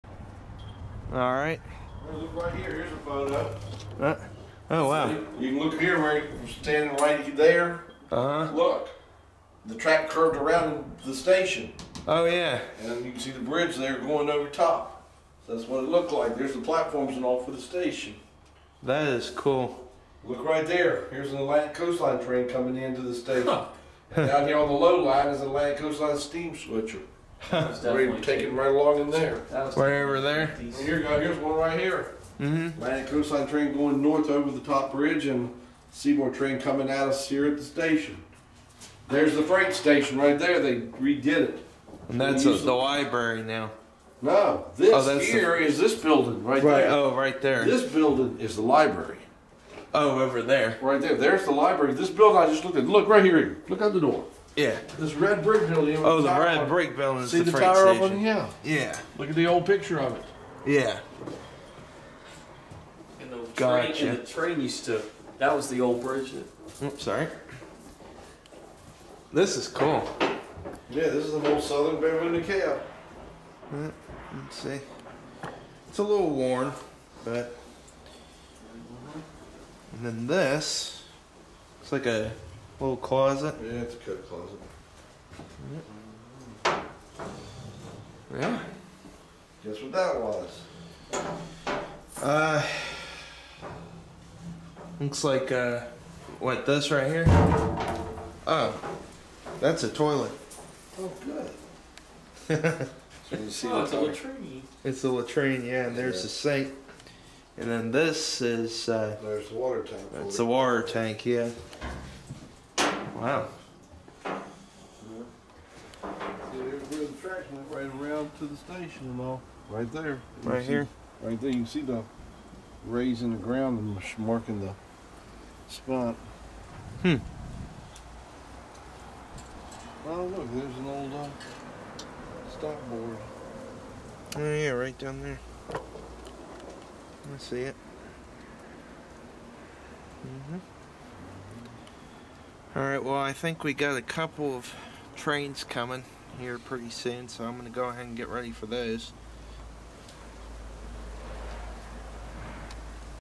Tour of a Southern bay window caboose!
Went in this vintage Southern bay window caboose and really liked what we saw. This video features a little of Barry's sarcasm...lol. Shot in 4K with a Sony Handycam FDR-AX53.

Recorded 1.2.2020